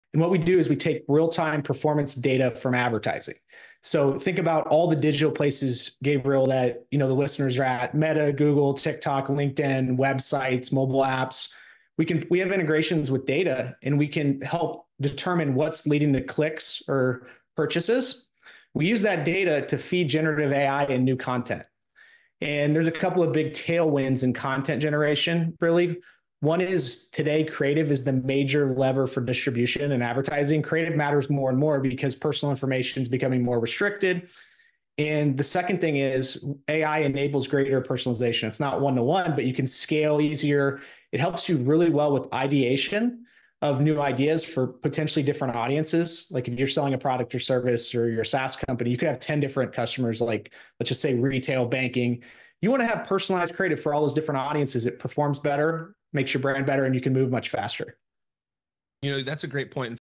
Desire: Learn from Omneky Business Officer
Join Omneky's Business Officer as he shares insights on AI marketing, branding, and personalized content for entrepreneurs and co-founders. Learn from a visionary in the industry!